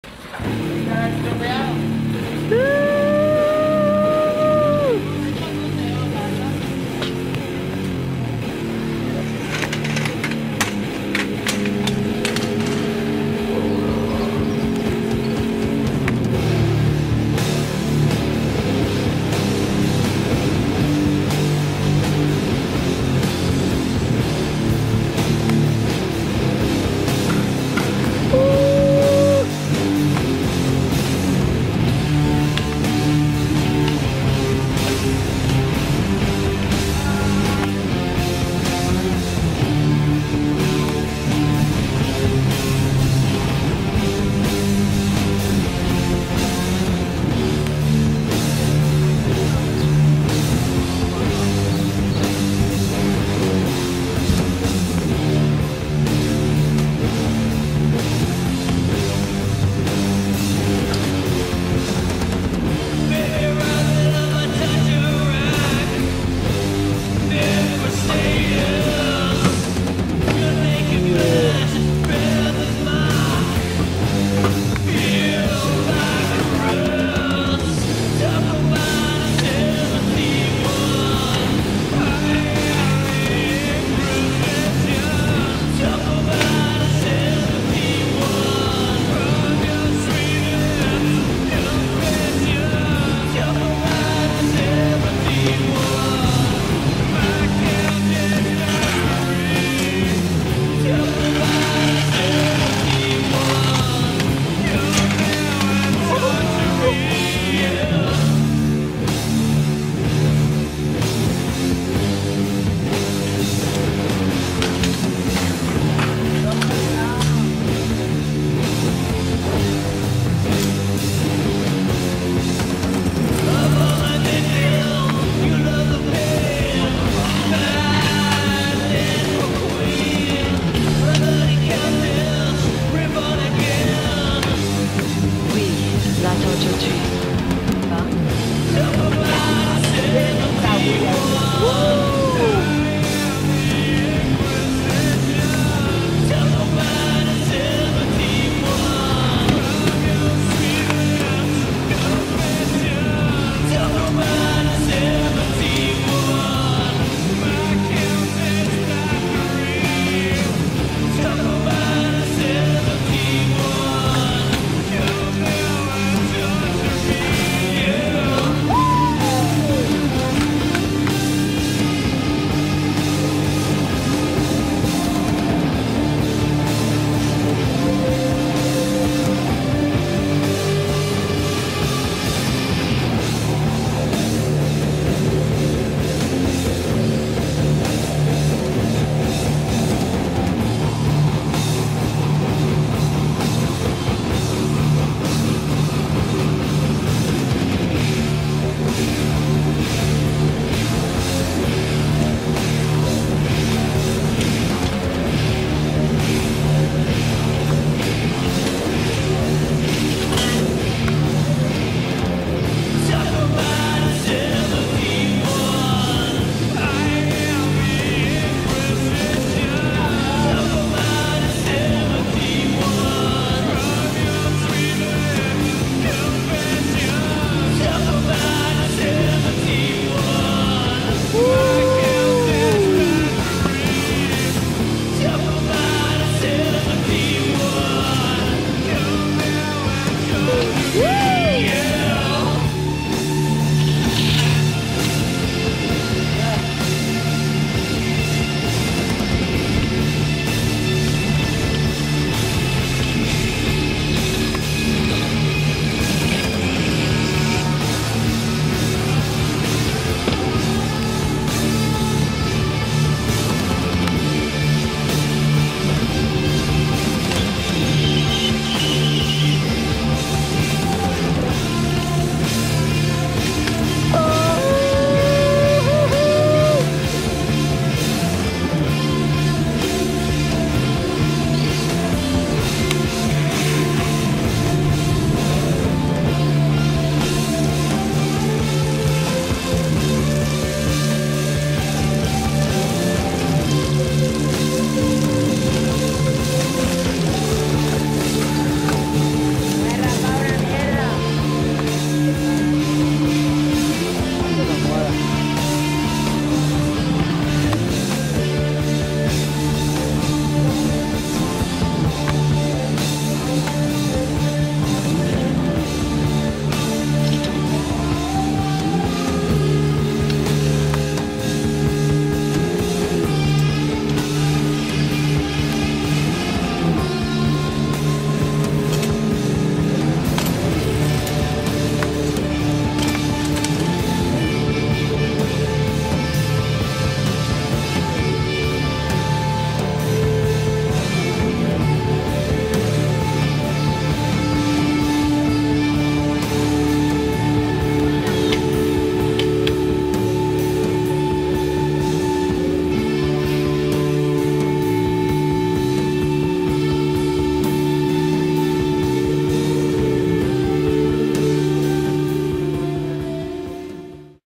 Torquemada 71 - SHRED TILL U DIE
RAULRA FILM ALL THE CLIPS WITH ONLY ONE BEARING IN THE FRONT WHEEL !! WE DON'T HAVE MONEY BUT WE HAVE BALLS
SHRED TILL U DIE